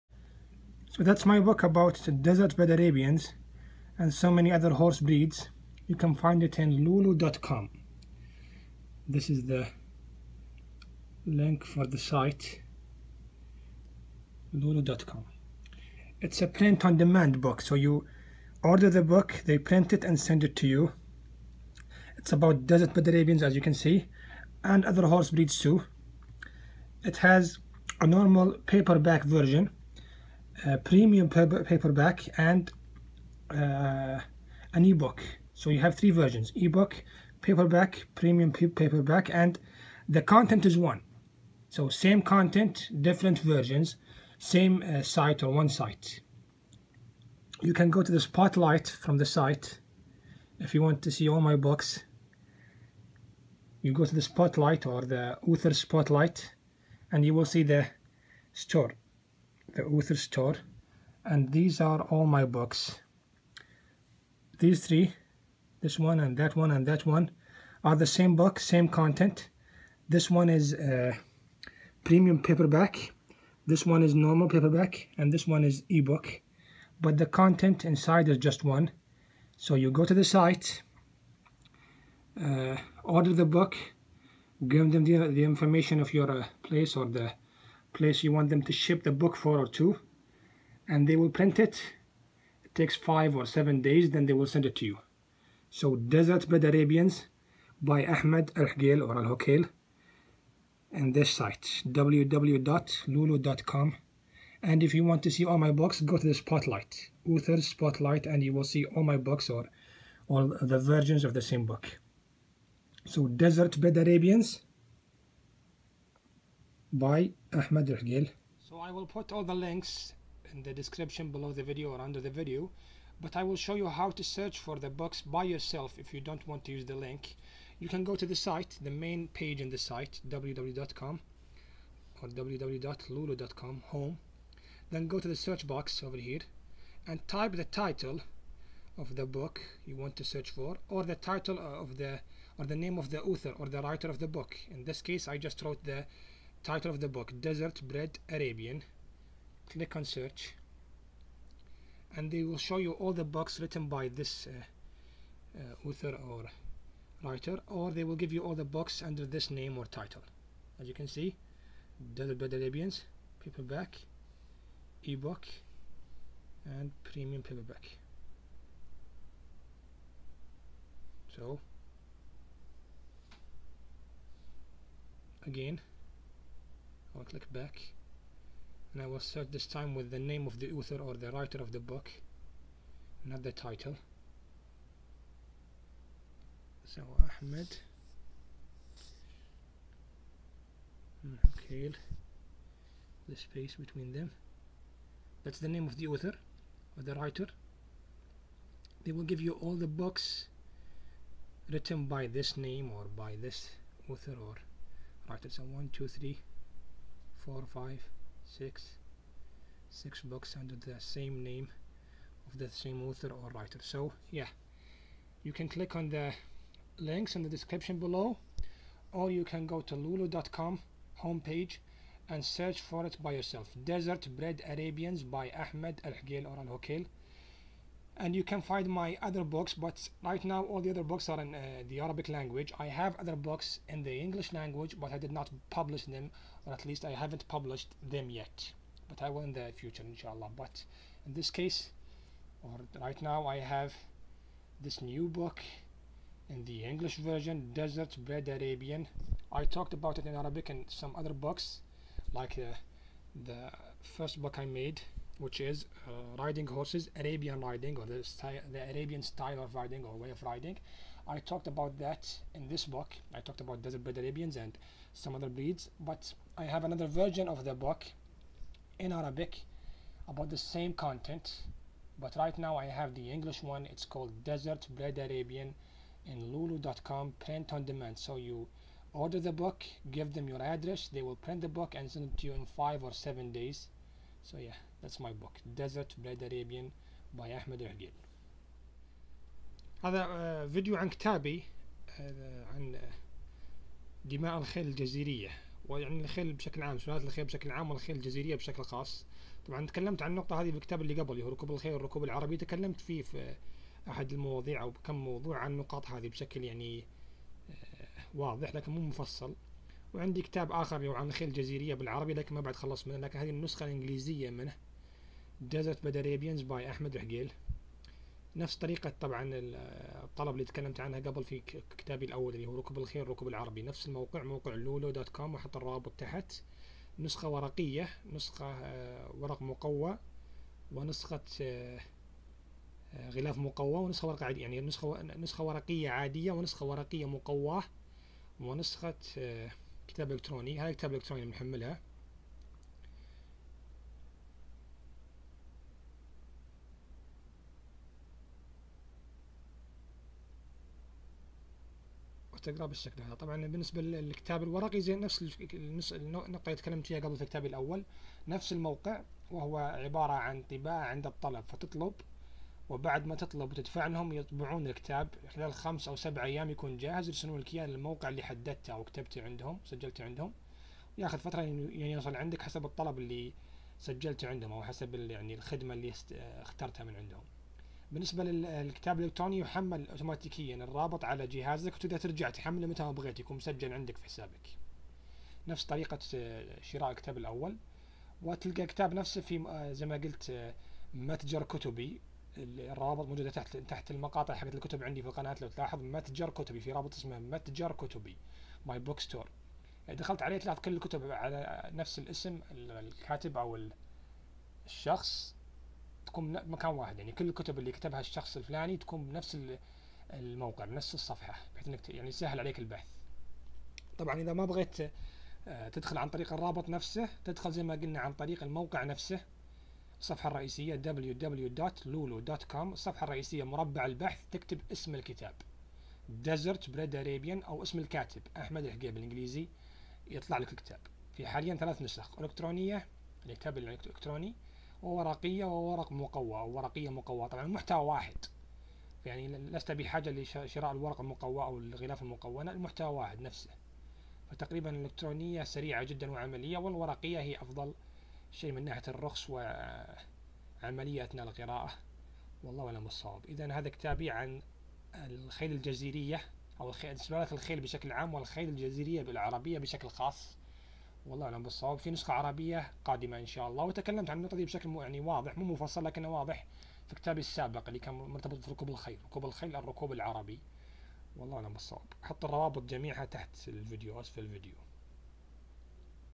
My Book : Desert Bred Arabian- by Ahmad Alhuqayl / كتابي عن الخيل العربية الجزيرية باللغة الإنجليزية
My book : Desert Bred Arabian / by Ahmad Alhuqayl

It's about Horse Breeds in general and The Desert Bred Arabian in particular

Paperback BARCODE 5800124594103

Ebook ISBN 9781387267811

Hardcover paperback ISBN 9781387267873

كتابي عن الخيل العربية الجزيرية باللغة الإنجليزية، تكلمت فيه عن سلالات الخيل بشكل عام والخيل العربية الجزيرية بشكل خاص وسبق أن تكلمت عن هالنقطة في كتابي السابق ركوب الخيل / الركوب العربي ويوجد نسخة عربية من نفس هذا الكتاب بالعربي لكنها قيد التنقيح حالياً.

متوفر حالياً في موقع لولو الإلكتروني لنشر الكتب.

BARCODE النسخه الورقيه الأولى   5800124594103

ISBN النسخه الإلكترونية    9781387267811

ISBN النسخه الورقيه المقواه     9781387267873

( ARABIAN RIDING ركوب الخيل / الركوب العربي )

Snapchat   ahmadalhuqayl